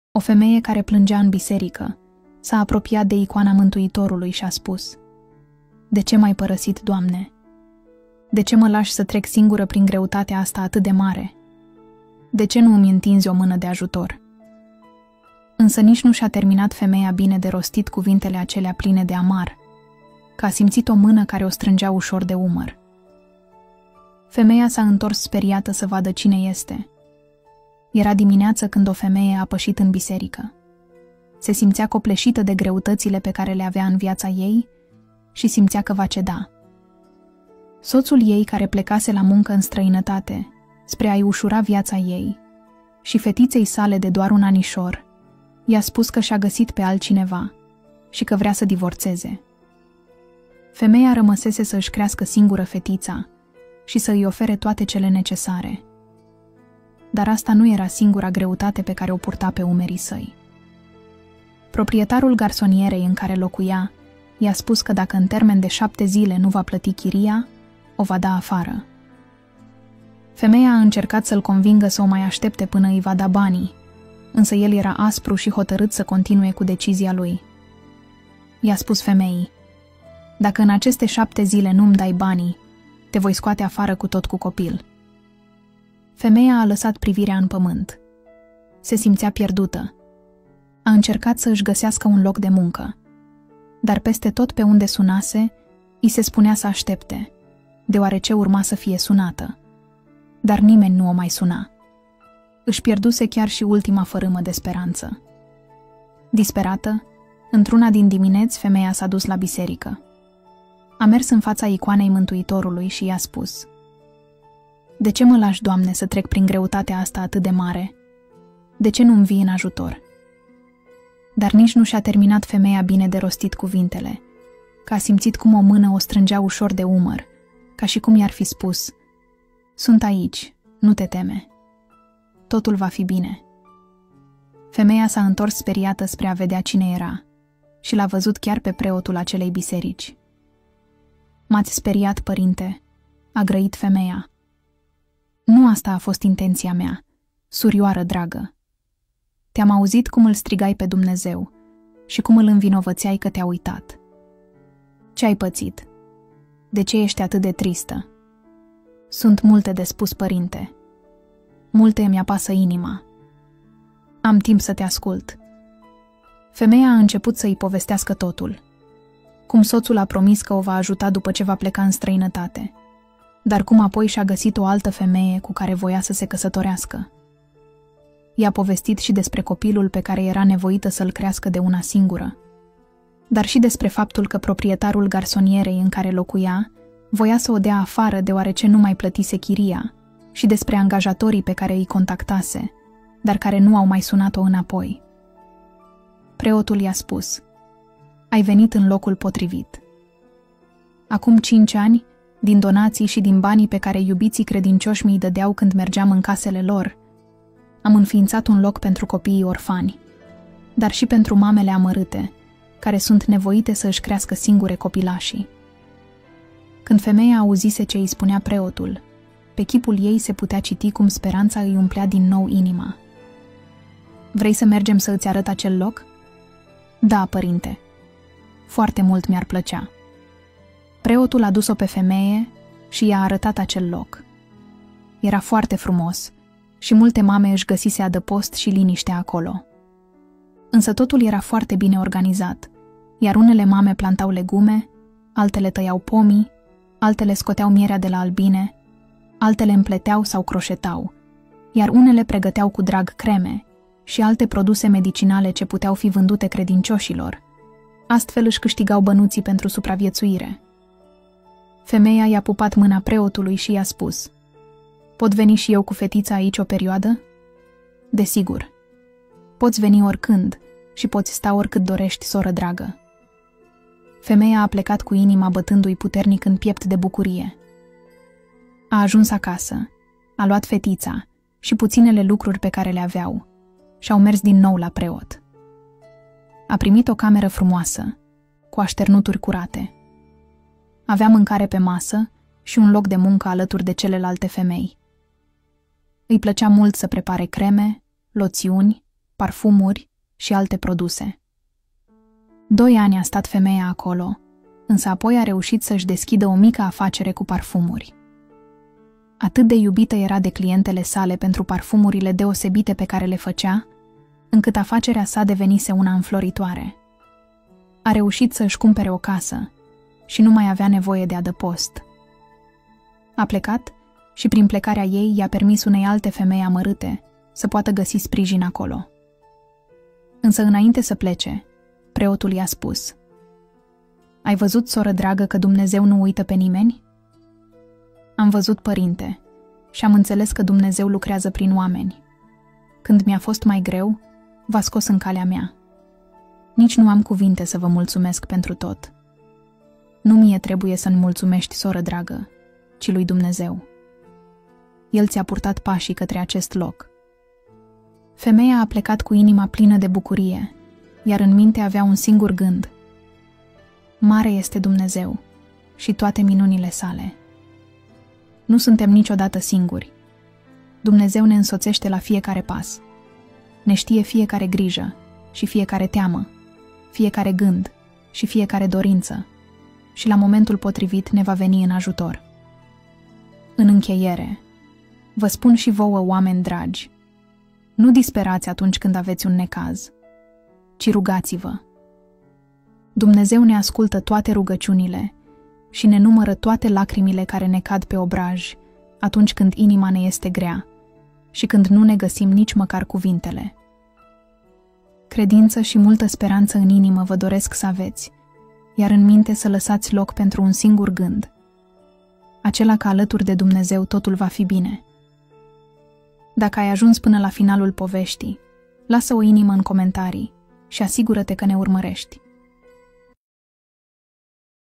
O femeie care plângea în Biserică, s-a apropiat de Icoana Mântuitorului și a spus:
-De ce m-ai părăsit, Doamne?  De ce mă lași să trec singură prin greutatea asta atât de mare?  De ce nu îmi întinzi o mână de ajutor?  Însă nici nu și-a terminat femeia bine de rostit cuvintele acelea pline de amar, că a simțit o mână care o strângea ușor de umăr.  Femeia s-a întors speriată să vadă cine este...